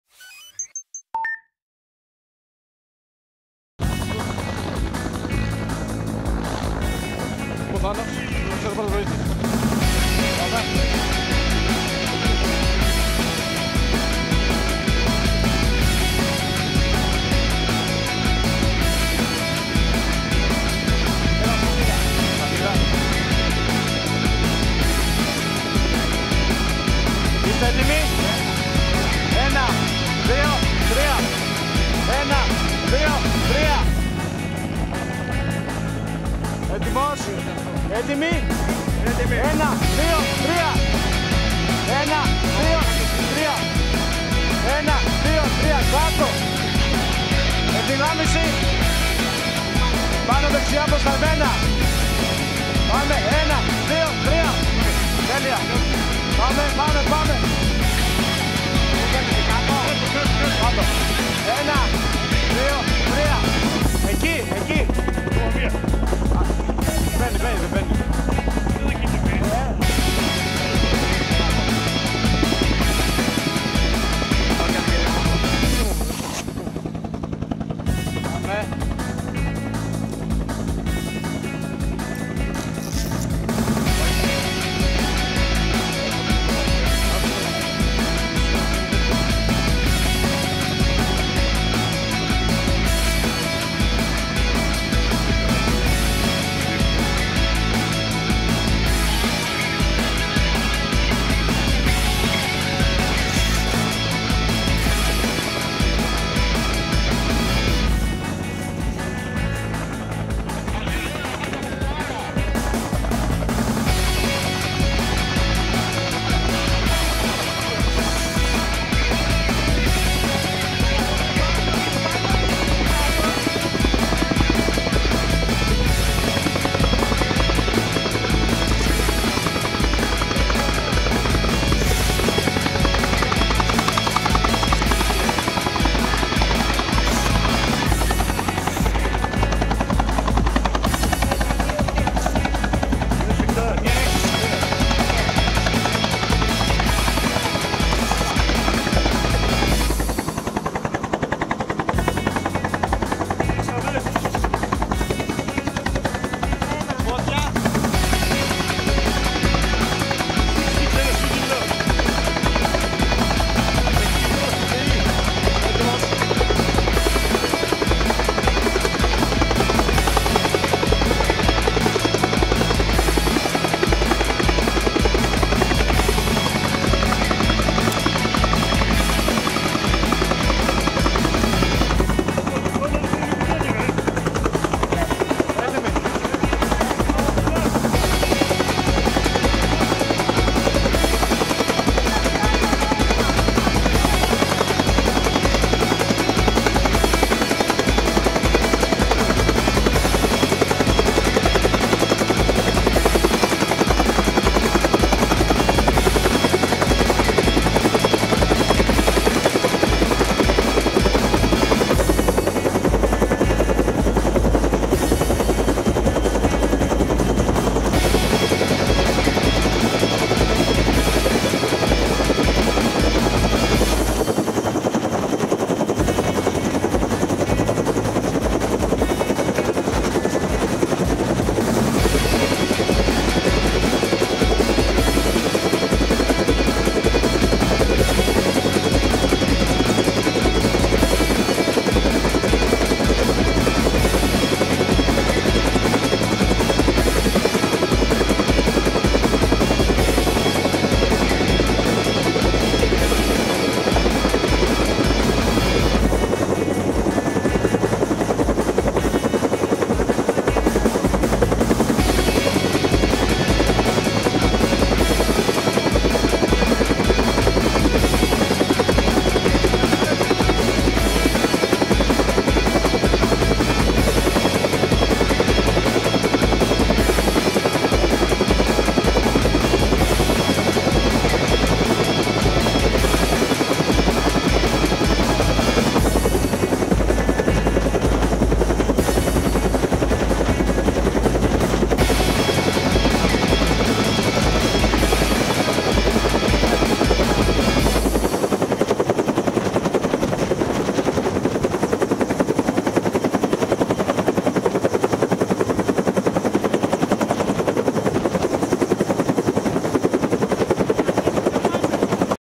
ΕΟΔ ΕΚΑΒ ΑΣΚΗΣΗ ΜΕ ΕΝΑΕΡΙΟ ΜΕΣΟ Ν ΑΓΧΙΑΛΟΣ 2017
ΔΙΑΣΩΣΤΕΣ ΑΠΟ ΤΗΝ ΕΟΔ ΚΑΙ ΤΟ ΕΚΑΒ ΣΥΝΕΡΓΑΣΤΗΚΑΝ ΣΕ ΑΣΚΗΣΗ ΜΕ ΕΝΑΕΡΙΟ ΜΕΣΟ ΚΑΙ ΦΟΡΕΙΟ (Ν.ΑΓΧΙΑΛΟΣ 2017)
VIDEO P.: thanos_skiathos